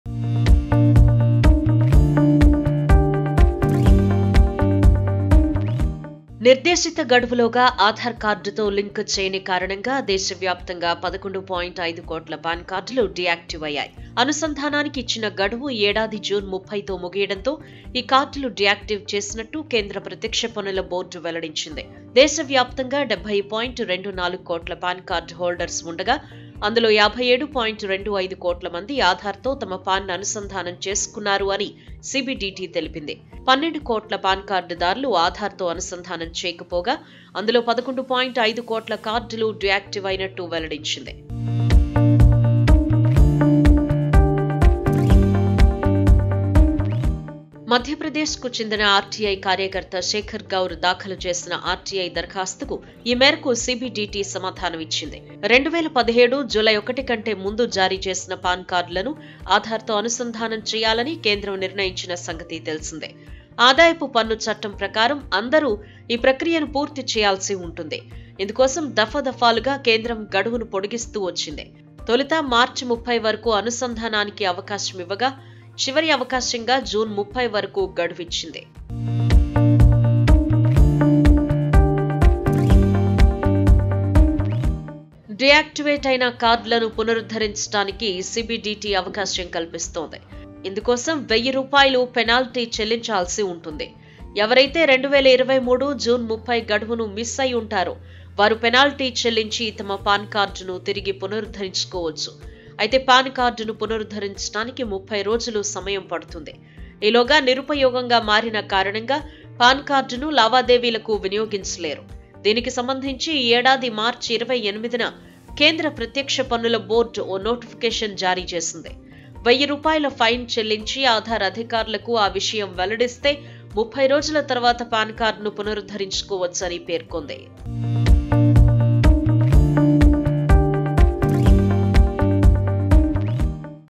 11.5 cr PAN cards deactivated for not linking with Aadhaar | 11.5 కోట్ల పాన్‌ కార్డులు డీయాక్టివ్‌
11.5 cr PAN cards deactivated for not linking with Aadhaar | 11.5 కోట్ల పాన్‌ కార్డులు డీయాక్టివ్‌  | ABN BIZTECH